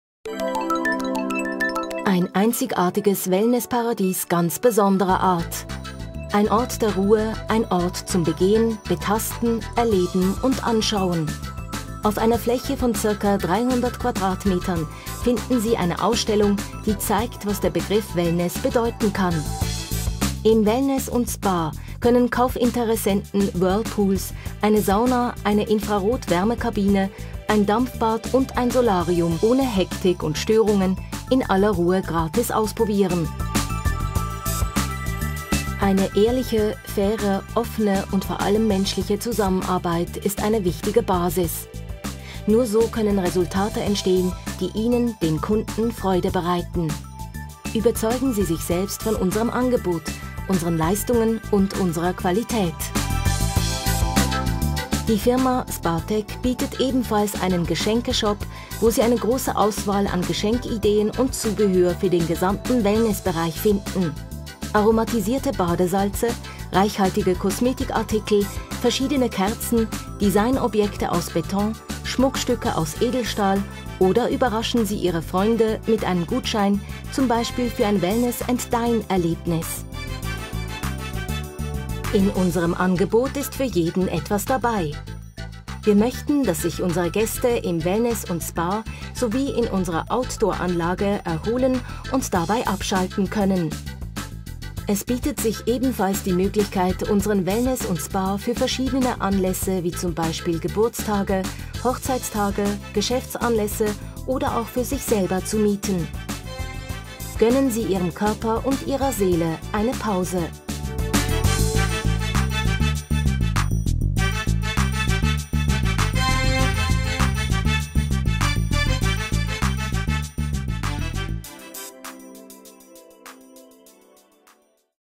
CITYGUIDE - Spatec Freizeit Anlagen GmbH Herisau Schweiz
Wellness bedeutet eine Auszeit vom stressigen Alltag zu nehmen und Körper, Geist und Seele gleichermaßen zu verwöhnen.
Erleben Sie eine Ausstellung, die zeigt was der Begriff "Wellness" alles bedeuten kann.